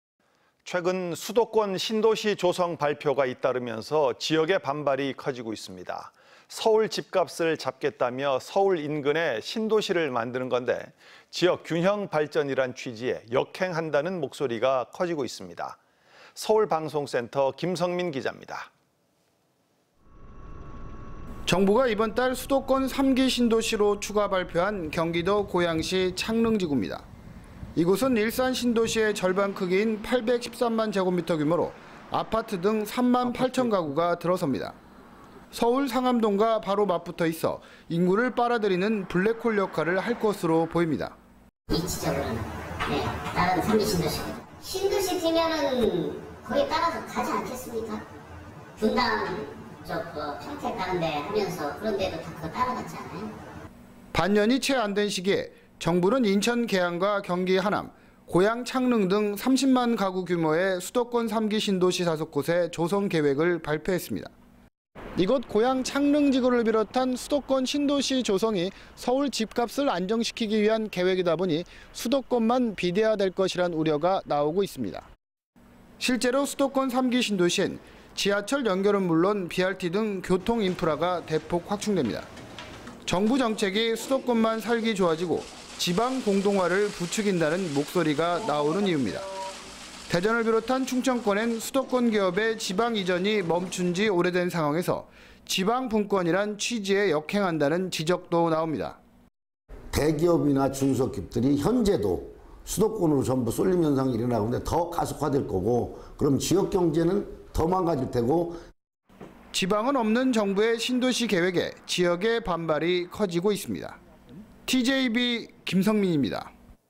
[TJB뉴스] 수도권 또 신도시.. 지방 공동화 부추기나?
최근 수도권
신도시 조성 발표가
잇따르면서
지역의 반발이

서울 집값을 잡겠다며
서울 인근에
신도시를 만드는 건데,
지역 균형발전이란 취지에
역행한다는 목소리가

서울방송센터
김석민 기잡니다.